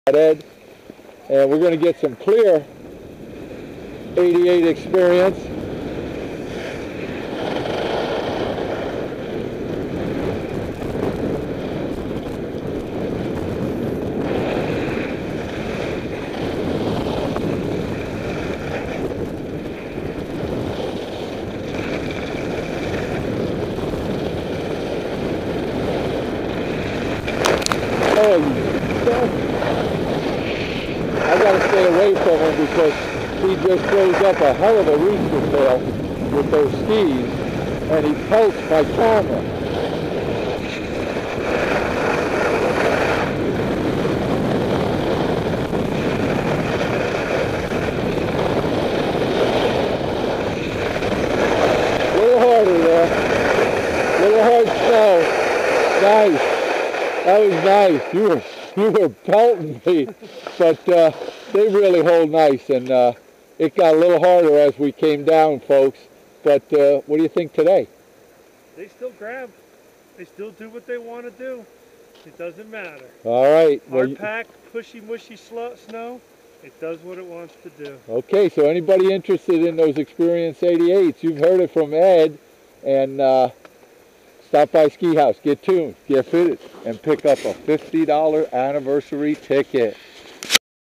Jan 24th The Big Man with a clear video of Ski Haus Ed on Rossi 88's.
Ed skiing new skies for the second day.